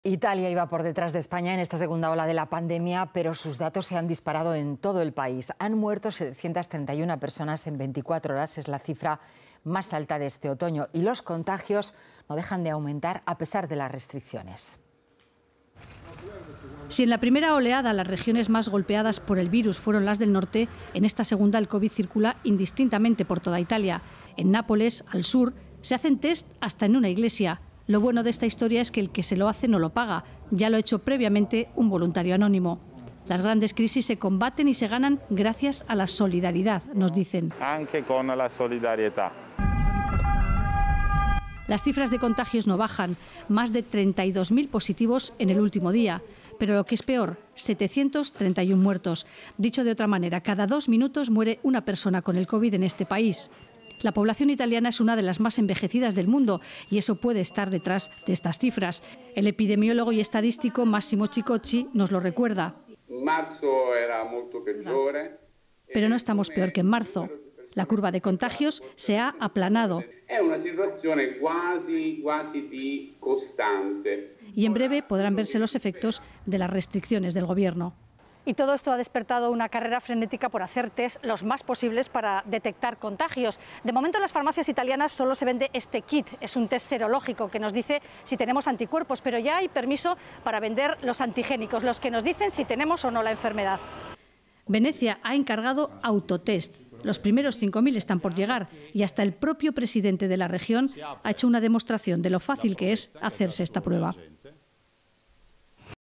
Se disparan los contagios en ITALIA a pesar de las RESTRICCIONES | RTVE Noticias
Italia iba por detrás de España en esta segunda ola, pero sus cifras se han disparado de Norte a Sur: Más de 700 muertos en 24 horas, el máximo de este otoño. Y los contagios no dejan de aumentar, a pesar de las restricciones.